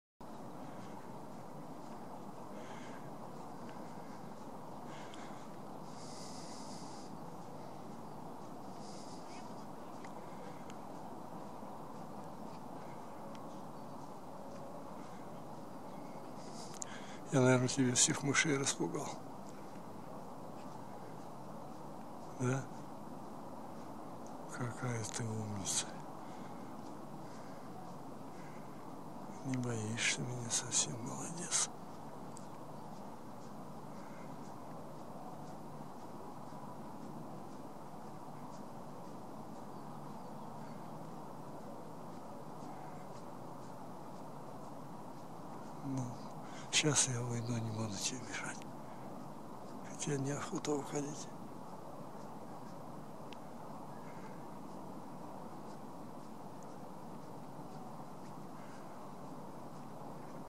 Бородатая неясыть-3.
Февраль 2017 года, остров Татышев что на Енисее в городе Красноярске.